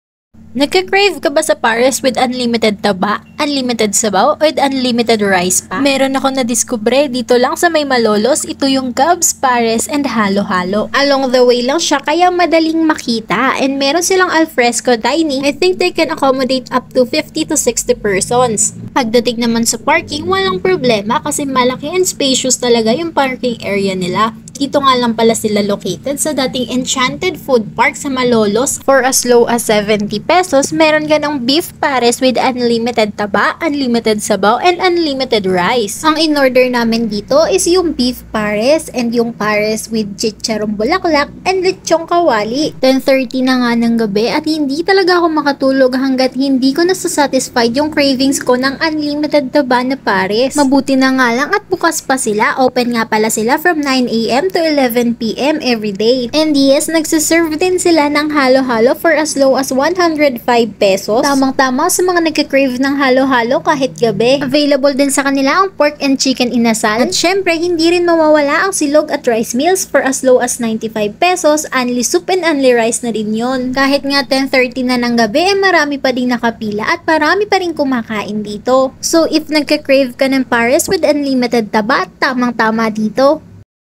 BAGONG PARESAN WITH UNLIMITED RICE, TABA, SABAW SA MALOLOS
Bagong kinakainan na Beef Pares with Unlimited Taba, Soup and Rice sa Malolos!